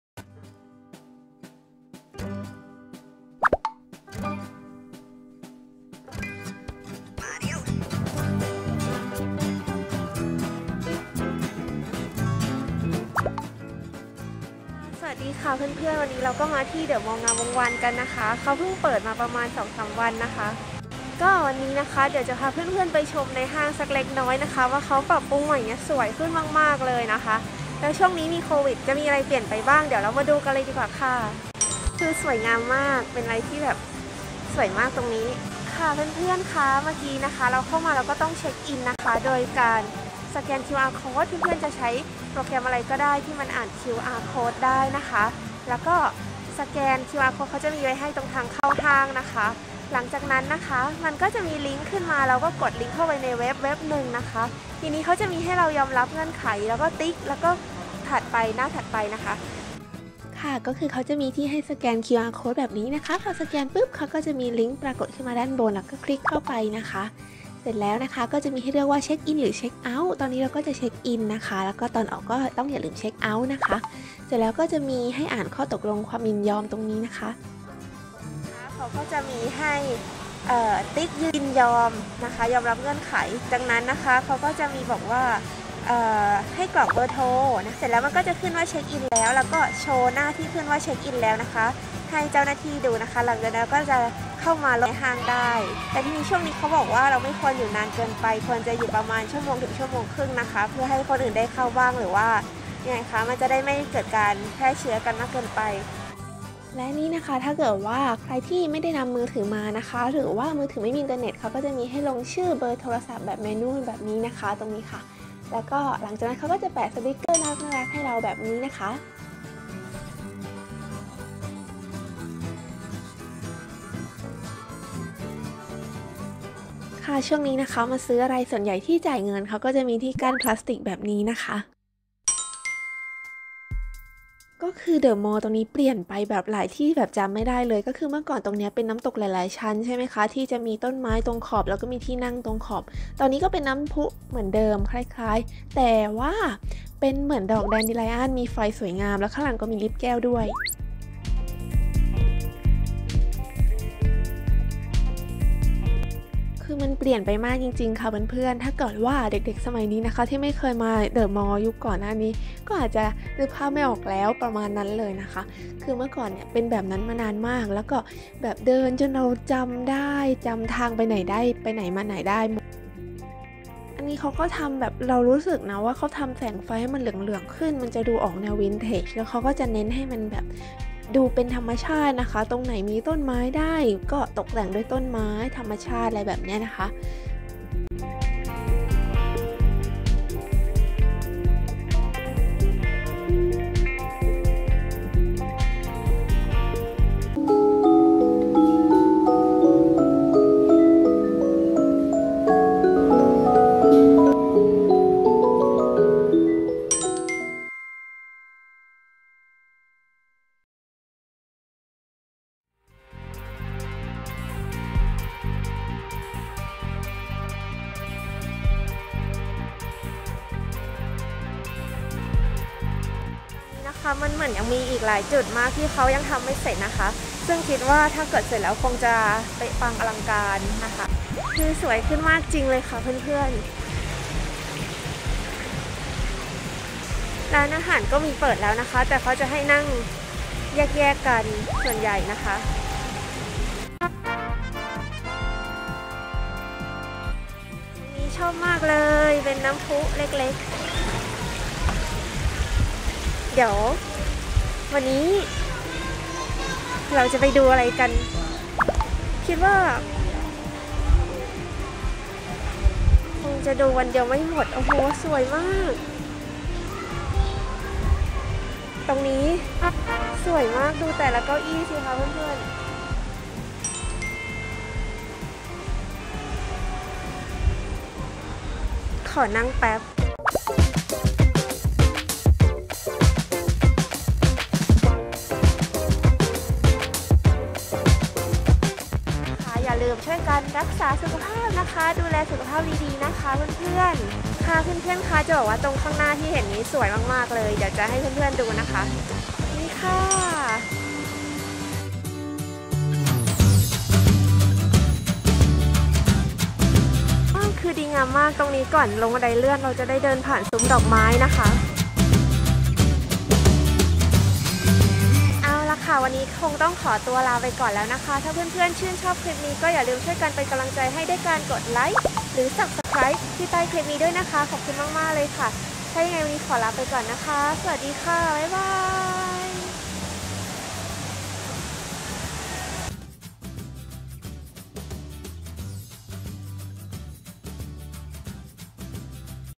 ไปห้างในยุคโควิด19 Scan QR Code, ใส่มาส์ก, social distancing เดอะมอลล์งามทำใหม่สวยมาก!! By Tangmelon
สวัสดีค่ะเพื่อนๆ วันนี้อยากจะพาเพื่อนๆมาชมการมาห้าง หลังจากที่ห้างเพิ่งเปิดอีกครั้งประมาณ 3 วัน เป็นกำลังใจให้ด้วยนะคะ อย่าลืม LIKE และ SUBSCRIBE น้าาาา ขอบคุณมากค่าาา

รักษาสุขภาพกันทุกคนนะคะ